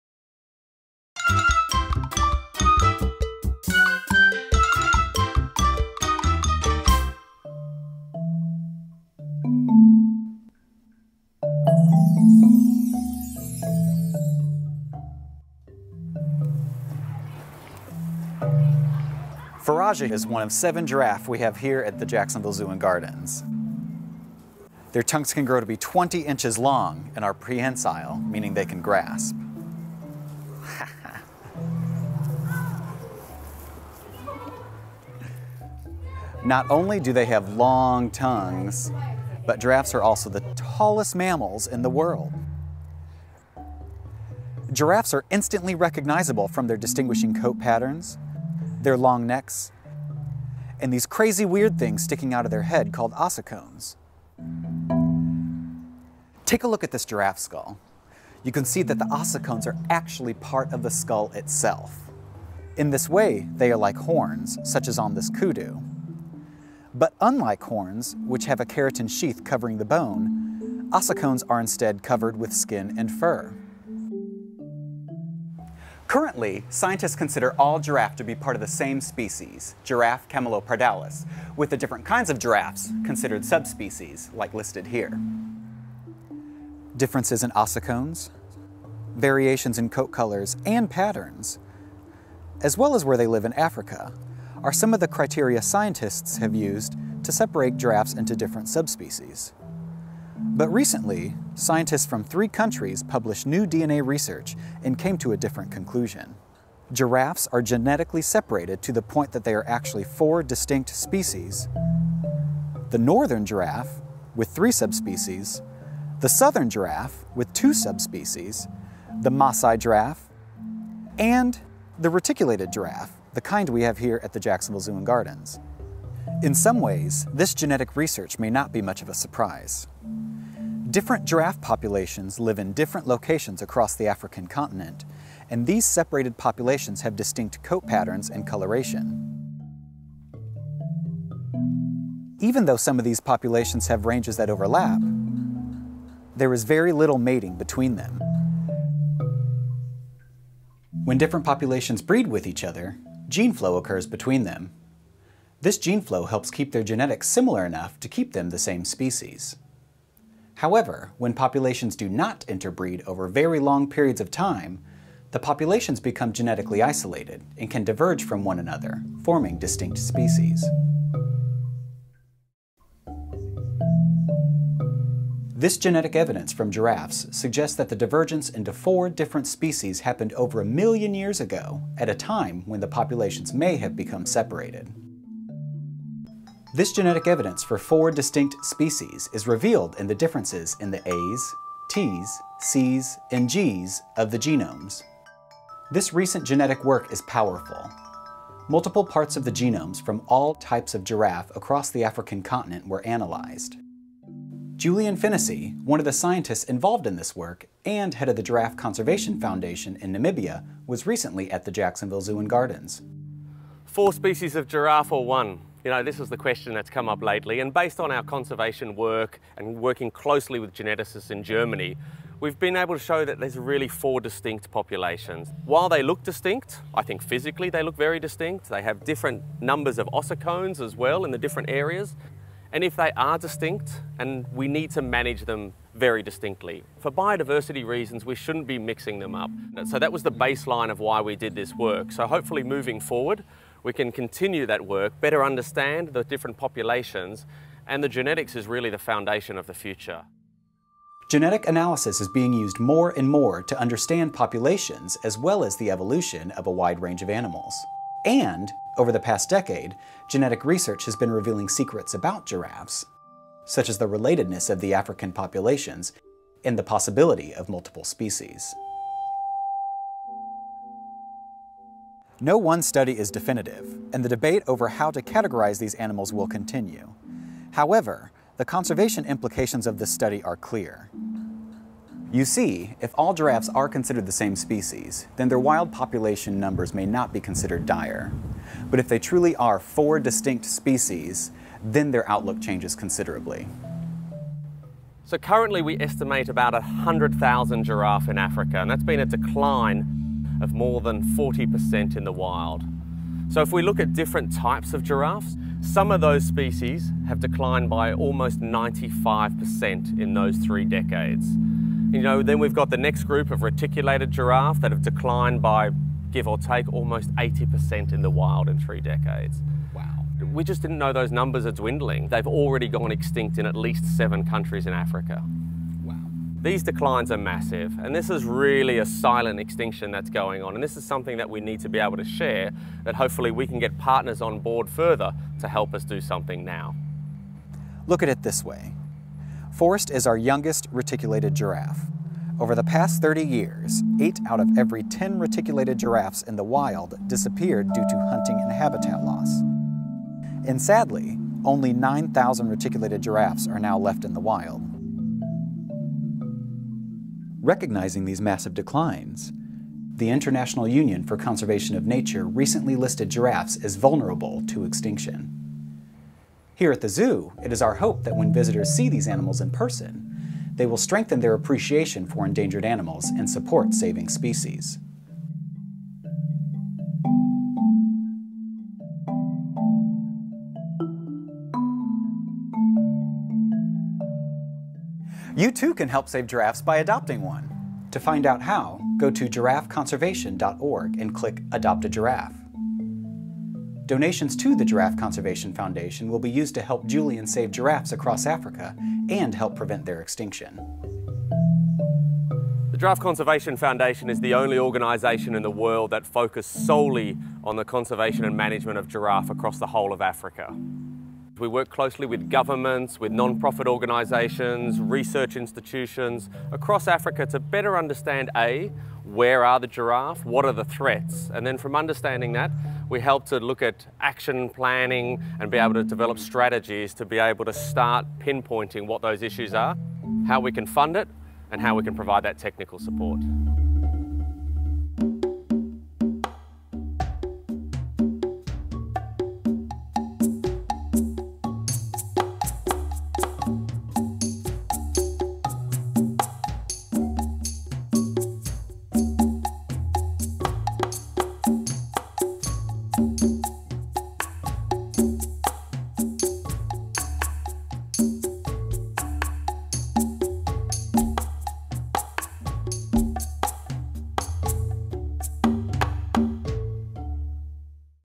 Endangered Giraffes... 4 species or 1?
Giraffe genetics, ossicones, coat spots and patterns, where endangered giraffes live, how many giraffe species there are, Giraffe Conservation Foundation, and Jacksonville Zoo and Gardens!  Join Faraja the giraffe, Jacksonville Zoo and Gardens Conservation Program Officer, Lucas Meers, and Dr. Julian Fennessy, Co-director of the Giraffe Conservation Foundation to learn about giraffes, their plight, and what you can do to help!